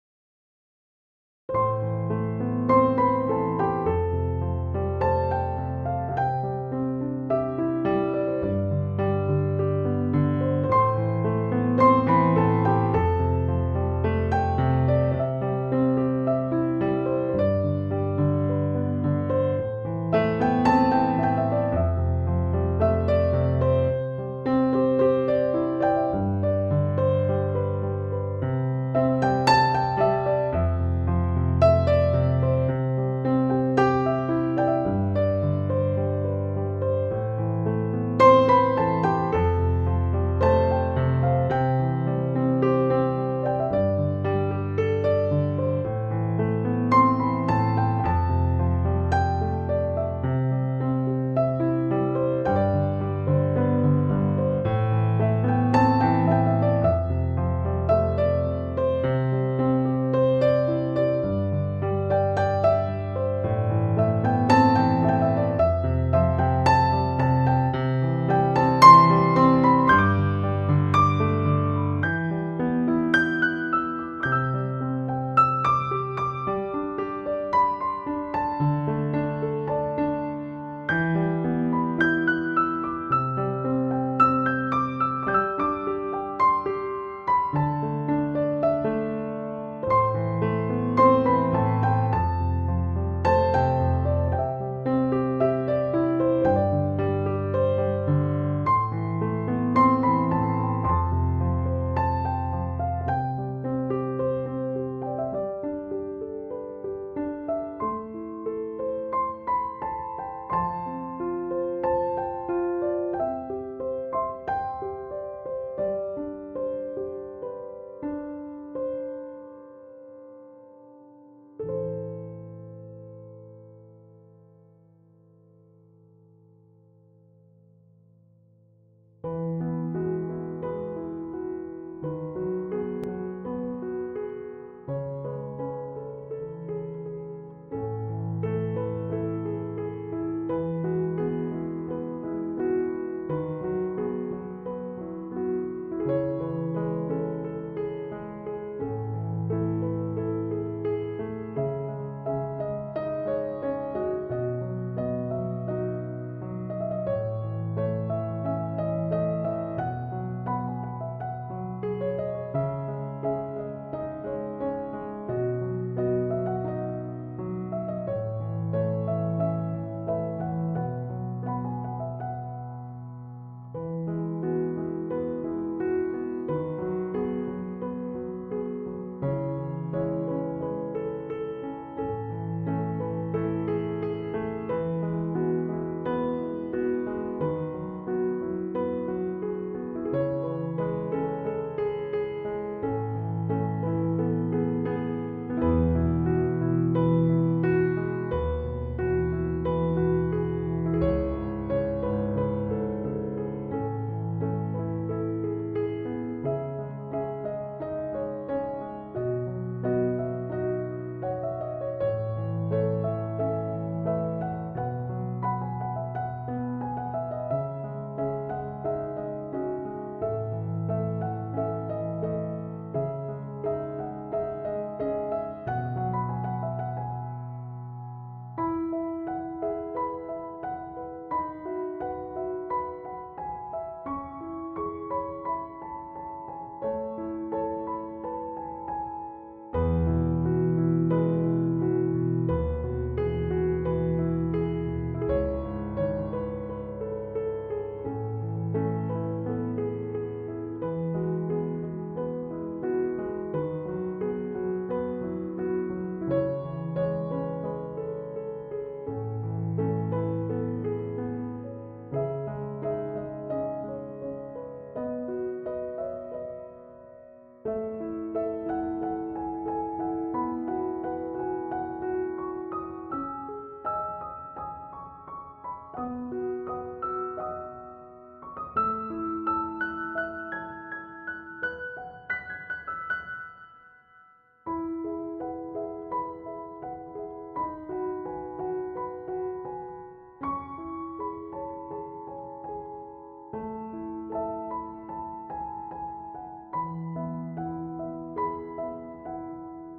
Sunset at home for mediation
In this video, we invite you to witness the breathtaking beauty of a sunset over the sea while being serenaded by the soothing sounds of background piano music. As the sun slowly descends, its radiant hues paint the sky with a stunning display of oranges, pinks, and purples, casting a warm and tranquil atmosphere over the serene waters. The calming notes of the piano music seamlessly blend with the peaceful ambiance, allowing you to sit back, relax, and take in the splendor of nature's artistry. Join us in this peaceful moment of reflection and let the sunset and piano music wash away the stress of the day.